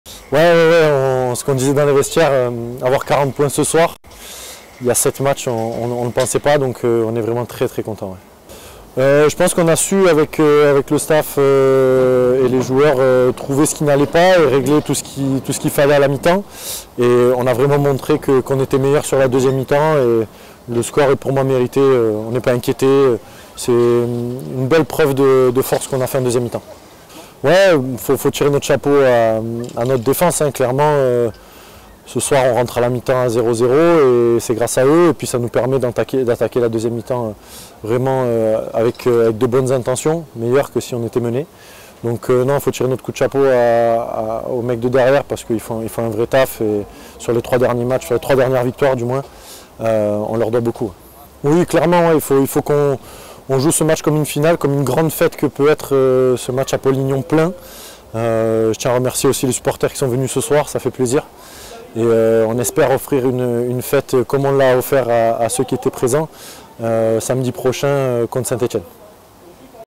J32 - RAF / FCA : Réactions d'après-match (Clément Depres)
Clément Depres s'exprime après la victoire de son équipe face aux Annéciens dans le cadre de la 32ème journée de Ligue 2 BKT.